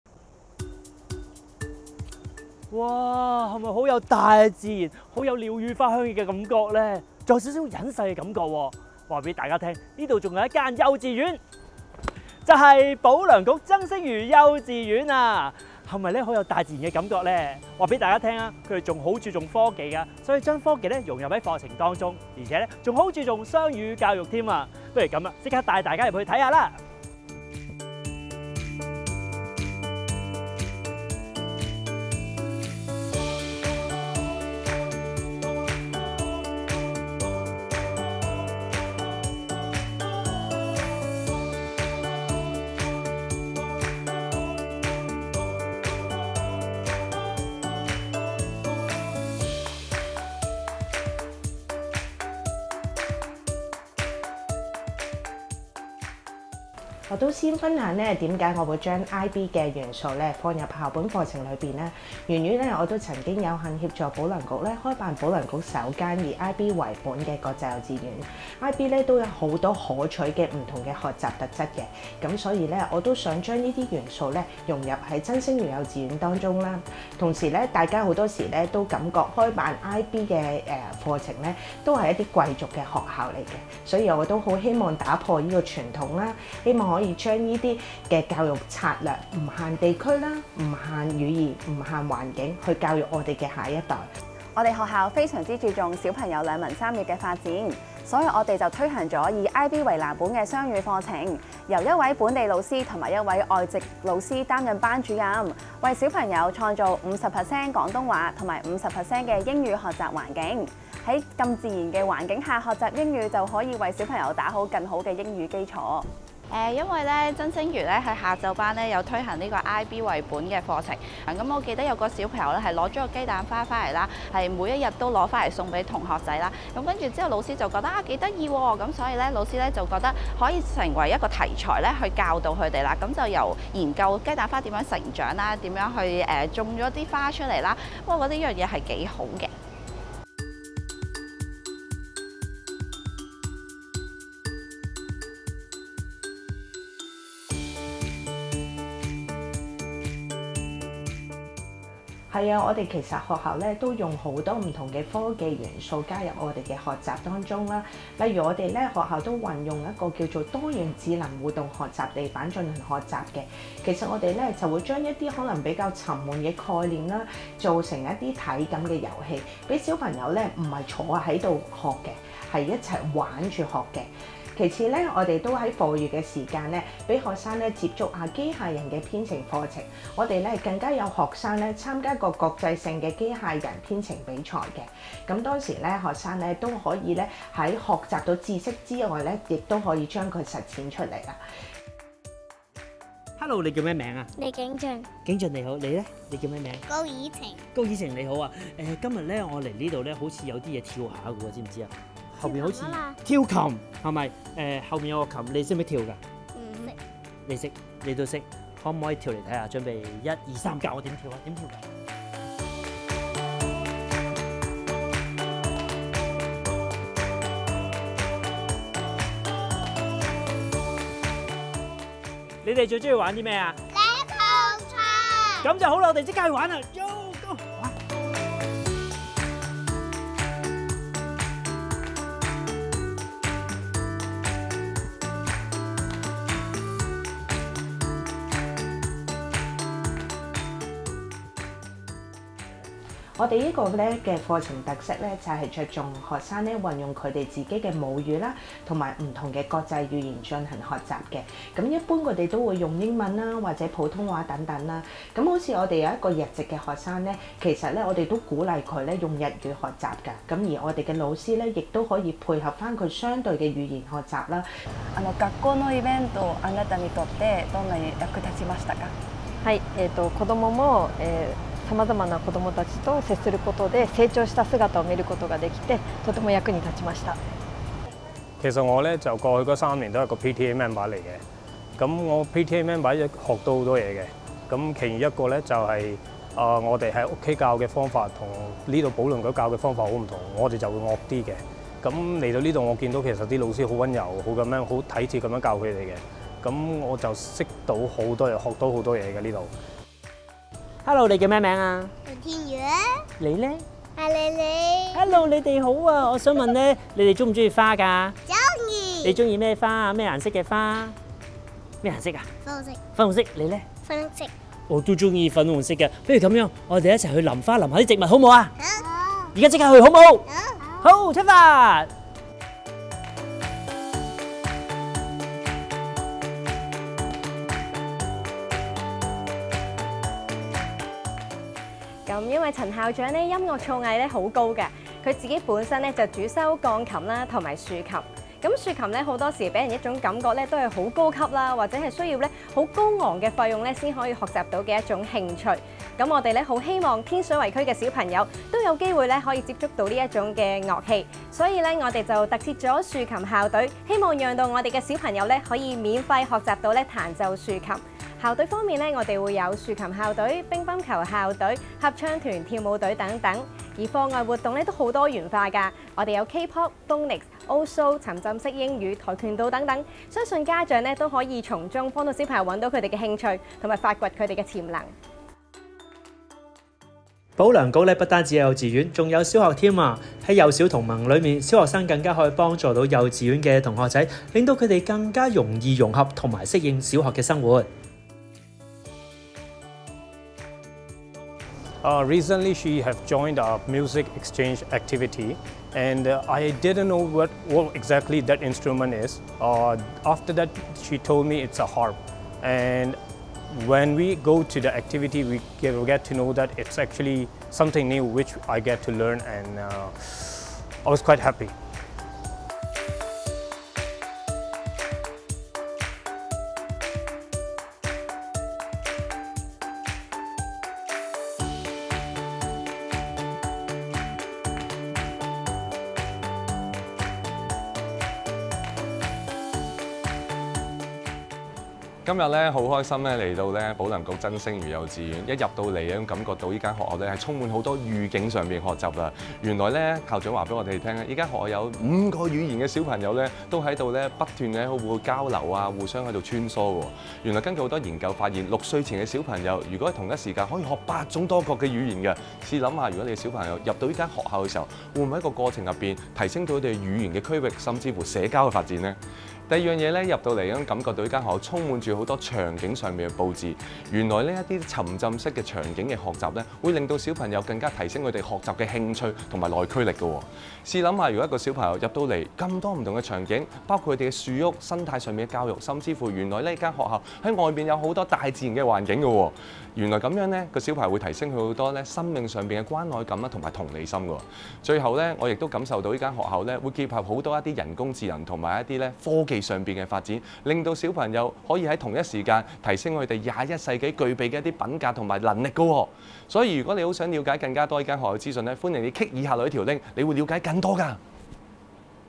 【爸爸媽媽必看！】「推行科技教學、雙語課程，建立幼兒國際性視野，培育正向思維，啟發幼兒無限潛能！」── 保良局曾星如幼稚園｜童您遊校100間｜伍文生｜小學｜幼稚園
文生哥哥帶各位家長走訪港九新界的幼稚園及小學，介紹不同學校的特色和理念！本集來到保良局曾星如幼稚園。校內提供一個關愛和諧及優質的學習環境，促進幼兒五育均衡發展，為社會培育人才，將所學回饋社會。採用藝術語文綜合課程，透過音樂、體育和美術等方式來學習語文，並提升創意、美感和體能的發展。有系統地推行「自由遊戲」，鼓勵幼兒按內在動機及興進行活動。其藉此發展創意、解難和社交的能力以建立自信心，且享追求快樂、挑戰和喜好的權利。致力推行「正向教育」，透過不同的渠道培養學生常存感思之心、珍惜一切，抱積極樂觀的人生觀，勇敢面對生活和成長的困難和挑戰。